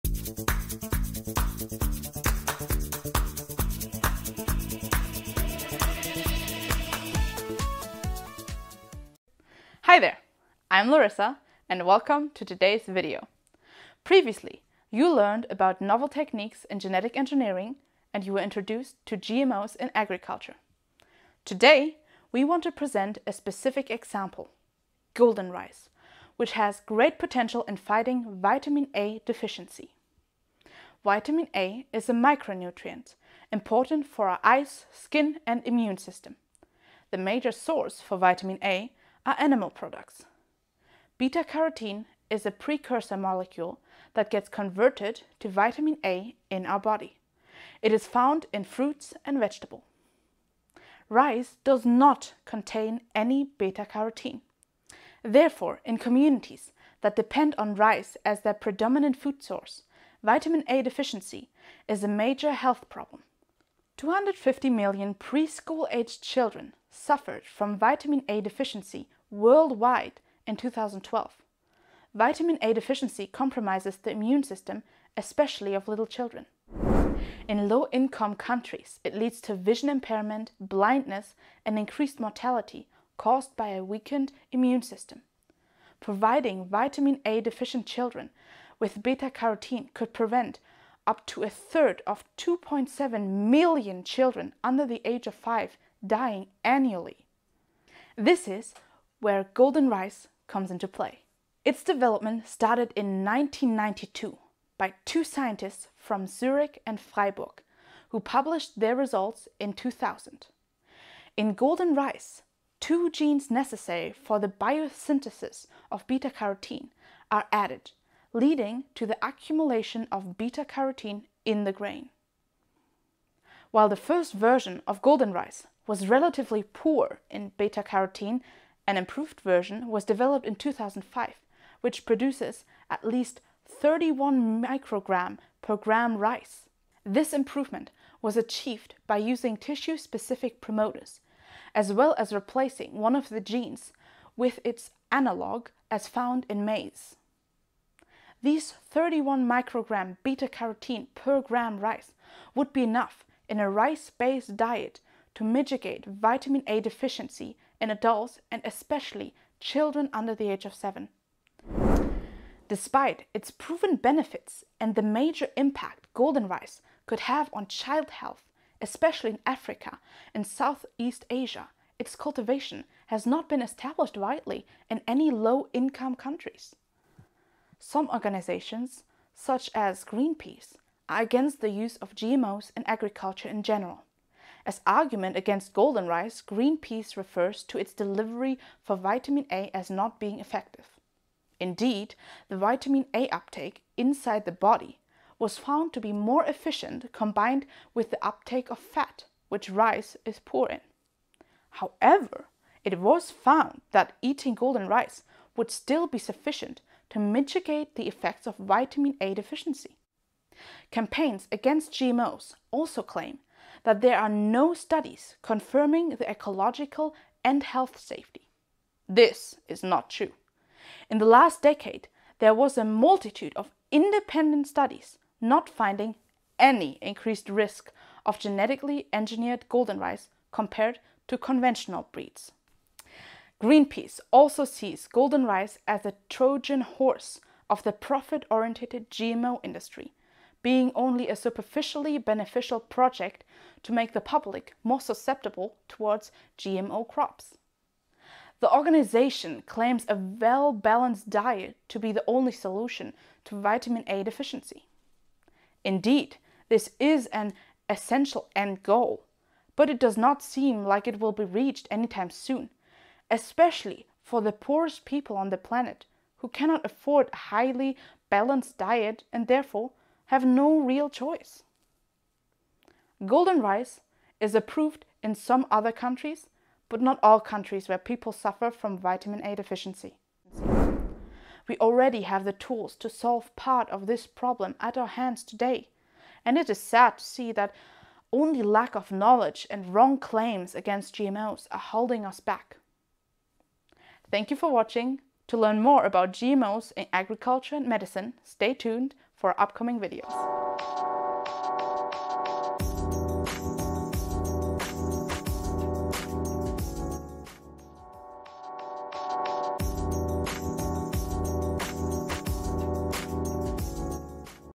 Geneducation#12: Golden Rice, an example of gene editing in agriculture
Hi there, long time no see!
Here is our finale geneducation video. After all the theoretical knowledge we have learned, it is nice to finish with a concrete case and see if what we know allow us to change our mind about GMO's potential.
Of course, this video doesn't try to justify profit oriented GMO industries.